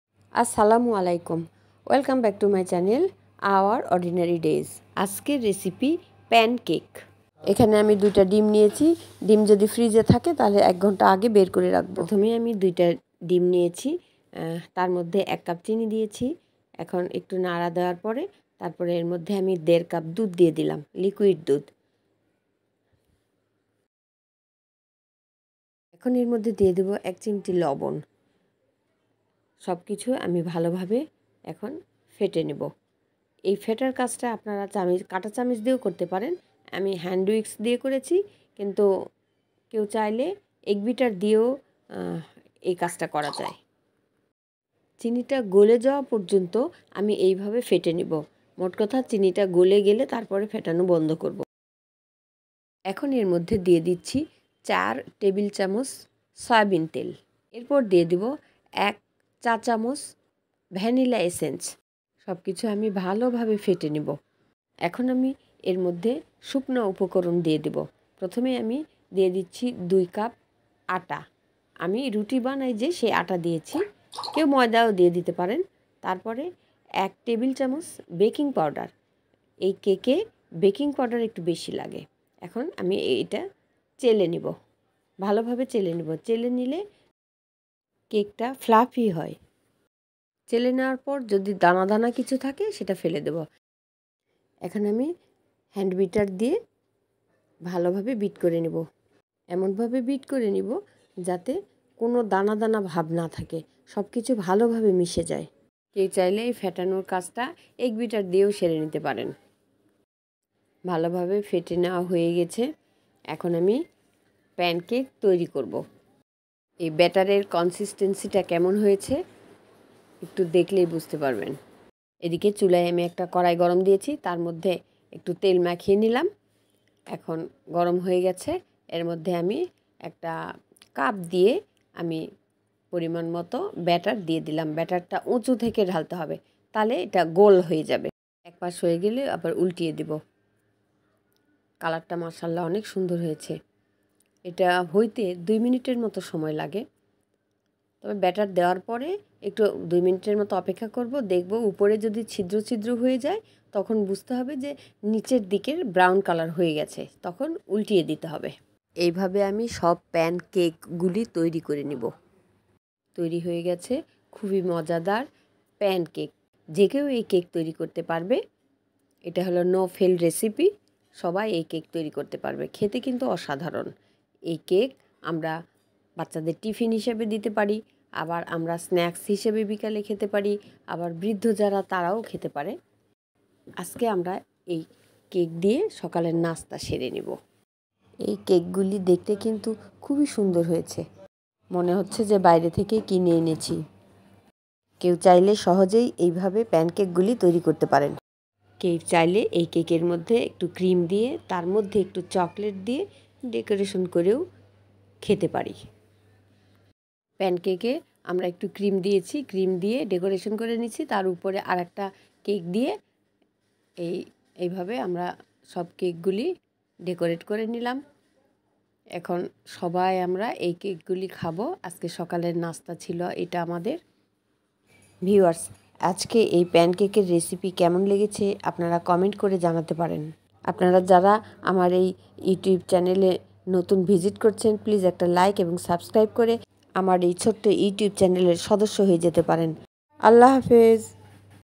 প্যান কেক রেসিপি || Pancake recipe
এই প্যানকেক দেখতে যেমন সুন্দর খেতেও অসাধারণ। হাতের কাছে থাকা মাত্র কয়েকটা উপকরণ দিয়ে এই প্যান কেক তৈরি করা । এই প্যানকেক বাচ্চাদের টিফিন হিসেবে, আবার আমরা স্ন্যাকস হিসেবেও খেতে পারি। এই কেক ছেলে বুড়ো সবাই পছন্দ করবে।